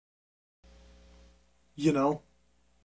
How to be Jim Root
In this video I explain in depth the inner workings of guitar legend, Jim Root